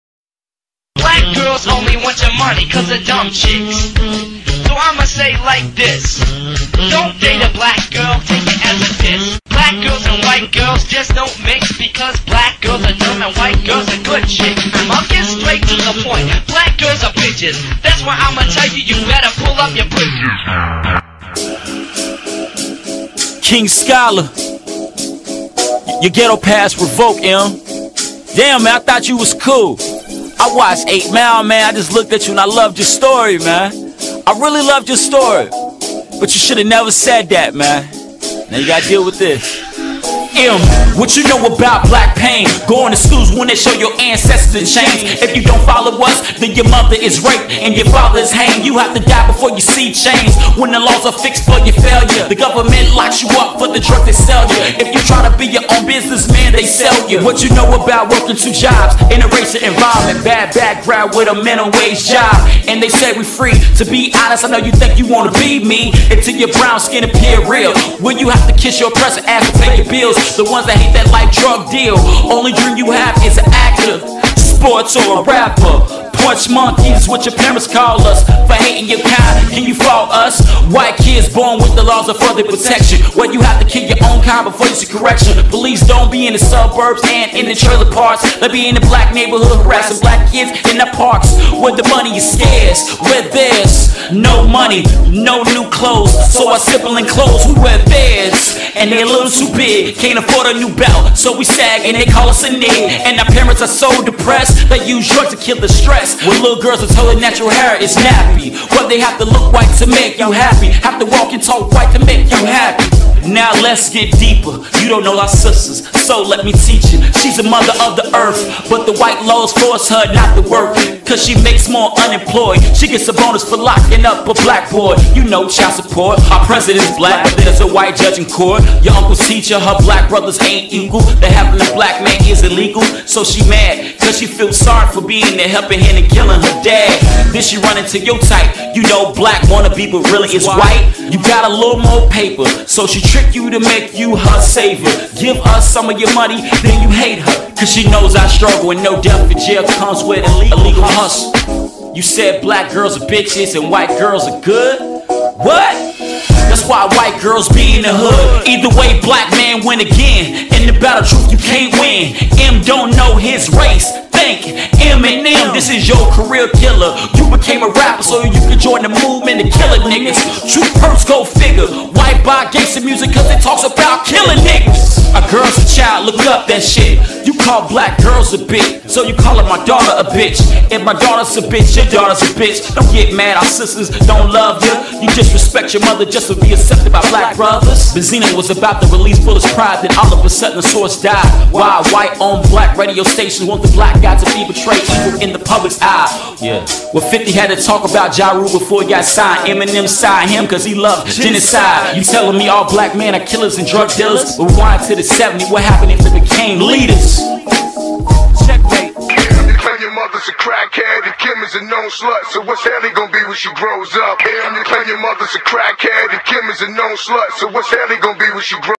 EMINEM DISS SLAVE SCHOLAR BLACK WOMEN PAIN
EMINEM DISSED BY LOUISVILLE KY EMCEE SLAVE SCHOLAR.